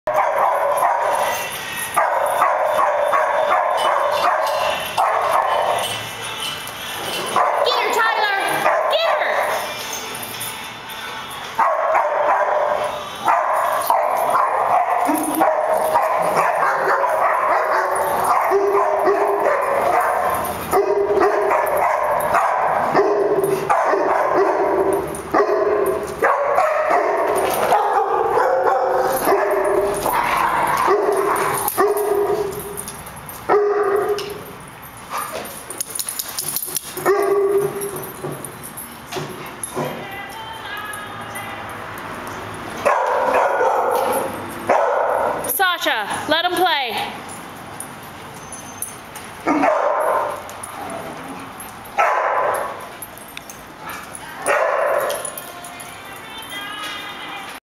Tyler and Savannah
Savannah (samoyed) tried for an HOUR to get Tyler (lab) to play with her, she flirted and kissed and bounced up and down and barked at. He just snubbed her and kept trying his best to ignore her. He finally came around though, and started flirting back and having fun.